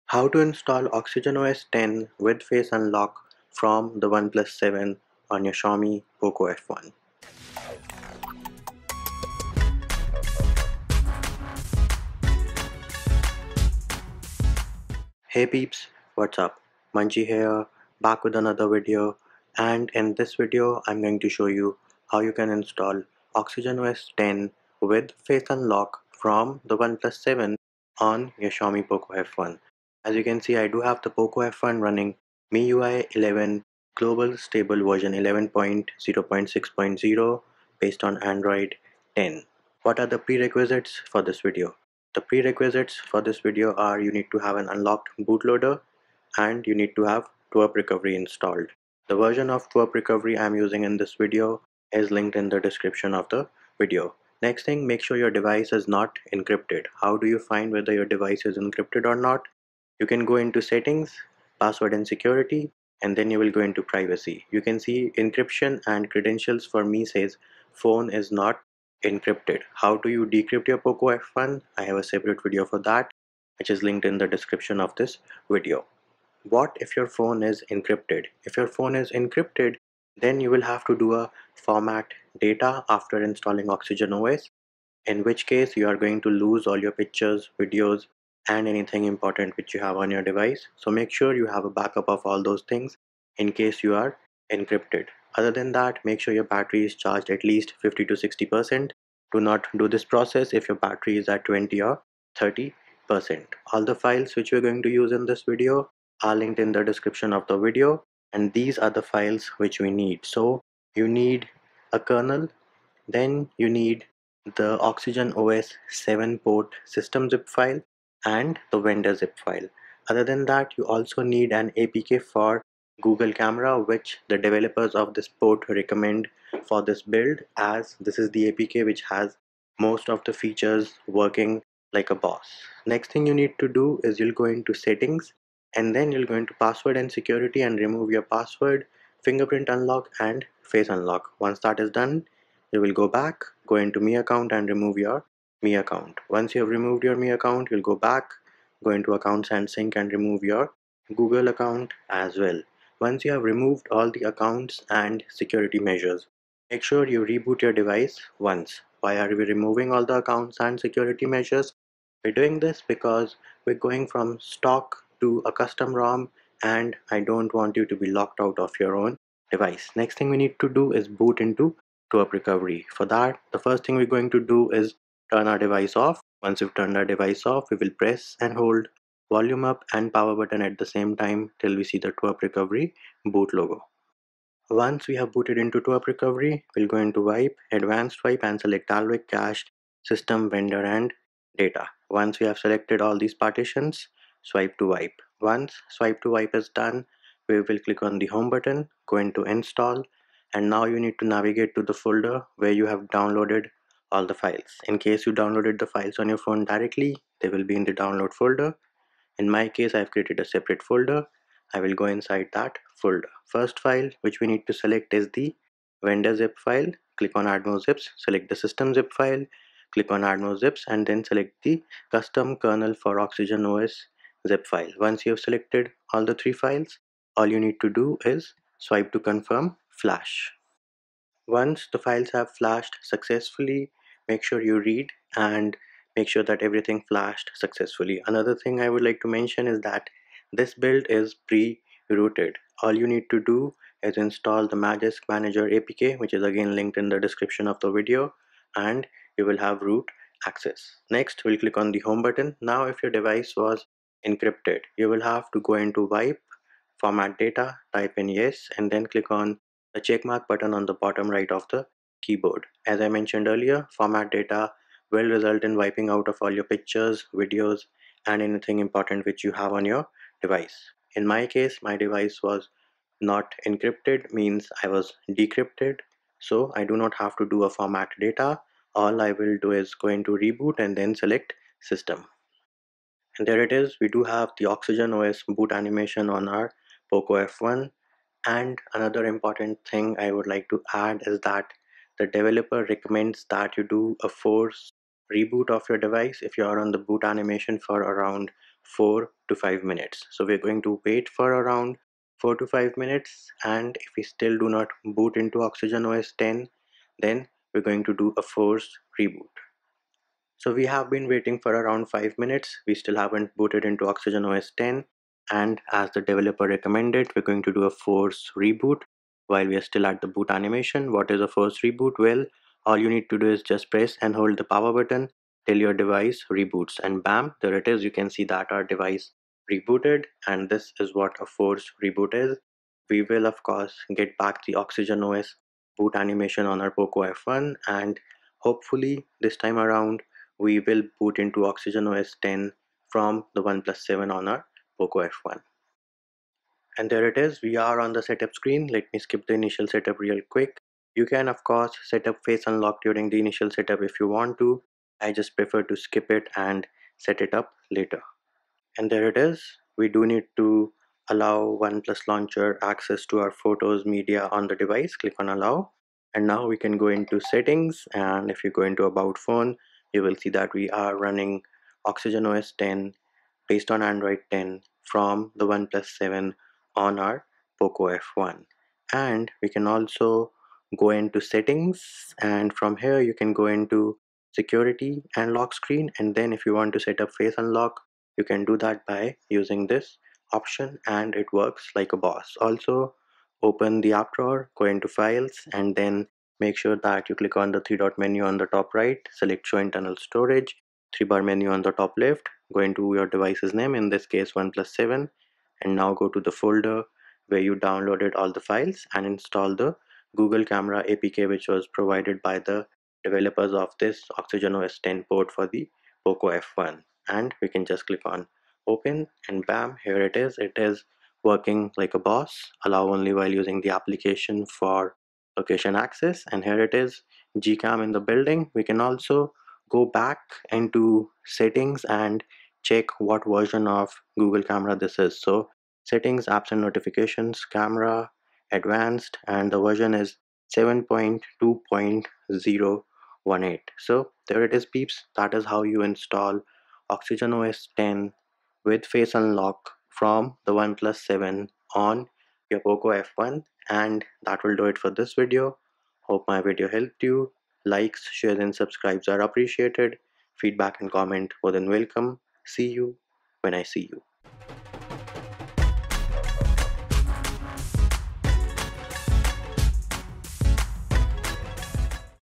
Poco F1 | Oxygen OS 10 With Face Unlock| Install & Setup | OnePlus 7 Port
Do you want to learn how to install Oxygen OS 10 with Face Unlock based on Android 10 from the OnePlus 7 on your Xiaomi Poco F1? In this detailed video tutorial we will show you how to install Oxygen OS 10 with Face Unlock based on Android 10 from the OnePlus 7 on your Xiaomi Poco F1.

**Poco F1 Oxygen OS 10 OnePlus 7 Port With Face Unlock Downloads**
If you are NOT on MIUI 11.0.6.0, please flash this firmware zip before vendor.
(Which kernel to use? Read pinned comment)